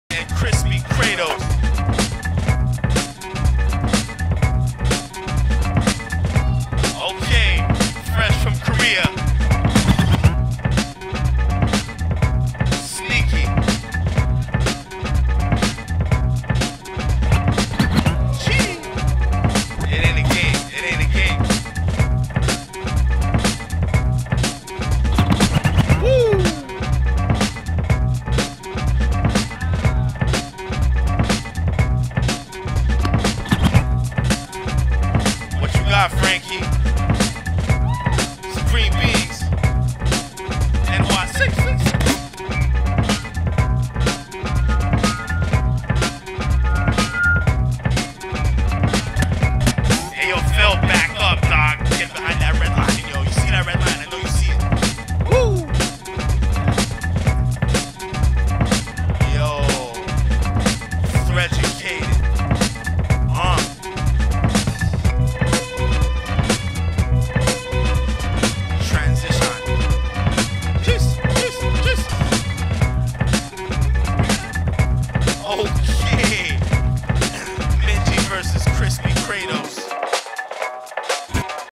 Rumble in the Dot 2016 - Phil VS Frankie [PRELIMS]
Prelims
Phil (Wizards; Vancouver) vs Frankie (Supreme Beings; NYC)

Rumble in the Dot
February 5, 2016
Luanda House, Toronto, Canada

DJ: Andy B. Bad
Band: Re.verse
Host: Switch B

Judges:
- Fil Fury | Floor Assassins Militia
- Prevail | Mighty Zulu Kingz | Ready To Rock
- Illz | Ground Illusionz | Lionz Of Zion